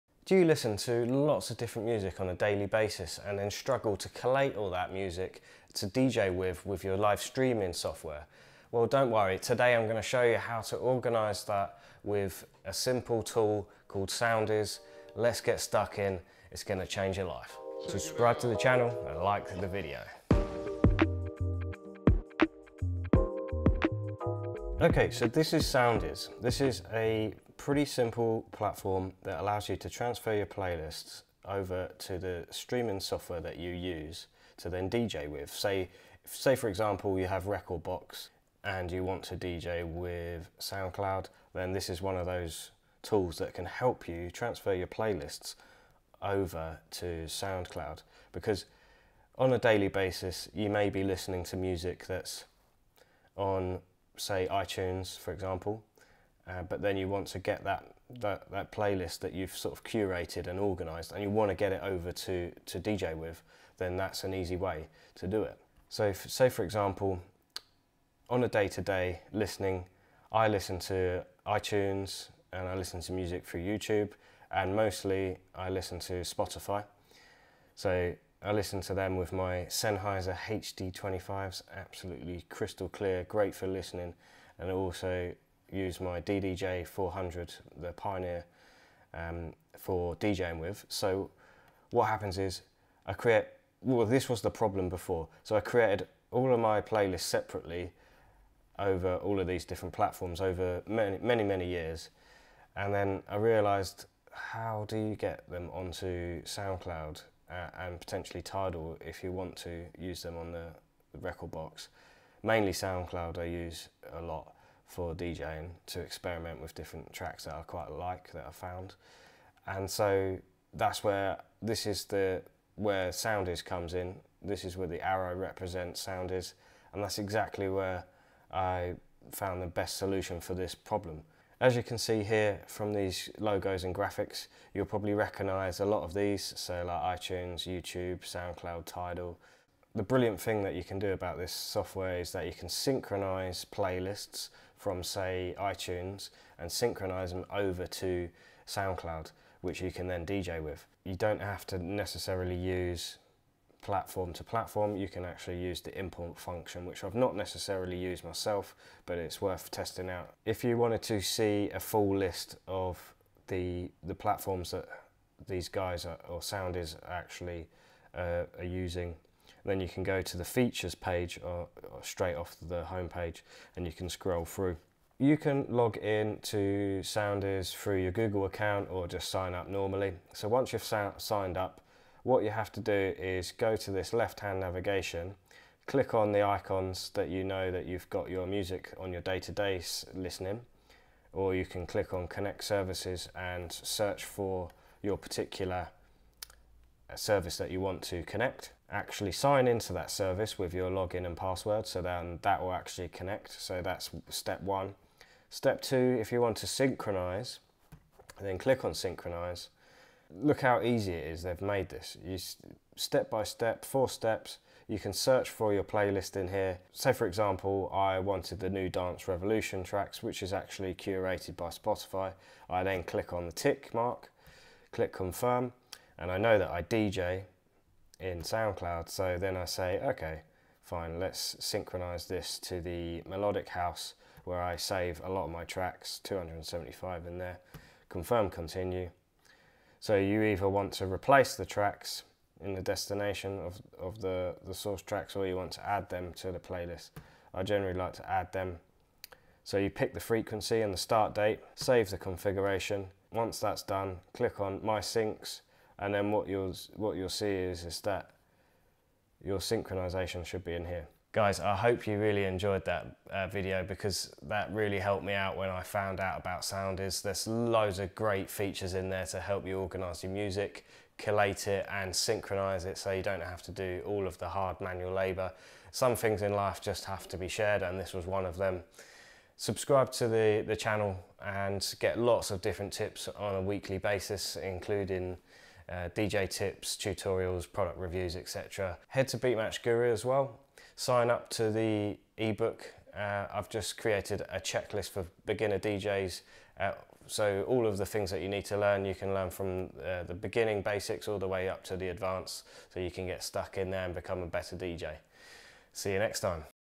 Organise DJ Streaming Music with Soundiiz
Learn how to organise and synchronise your DJ music playlists via connecting Spotify and other music platforms to Soundcloud and other DJ streaming platforms.

for more great DJ tips & tricks for beginners and intermediate DJs.